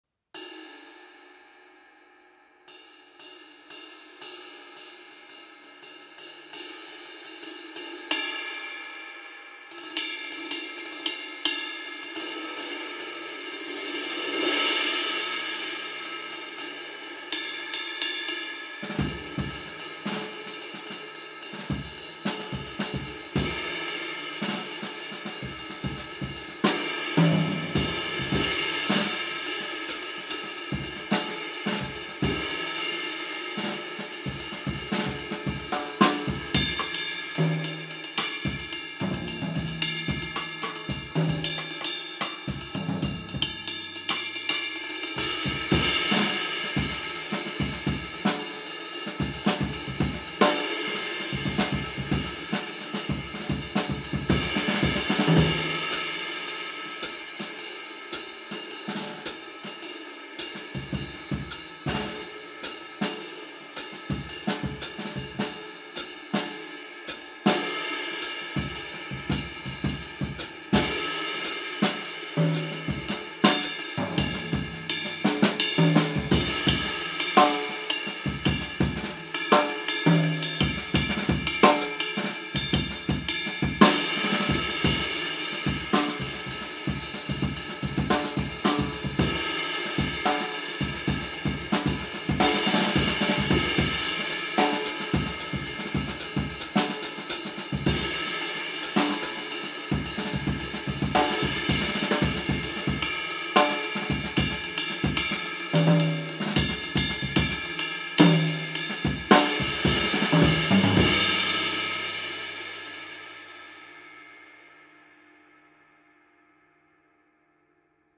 Quick Demo of Istanbul Agop Traditional Jazz 22" Ride
Kit: DW Performance 18-10-14" kit, Sonor Vintage 14X5.75" snare
Cymbals: Paiste 900 Sound Edge 14" hats (989/1124g), Istanbul Agop Traditional Jazz 22" ride (2352g)
Performer: Andrew Wyatt

Recorded with a Zoom Q2n 4k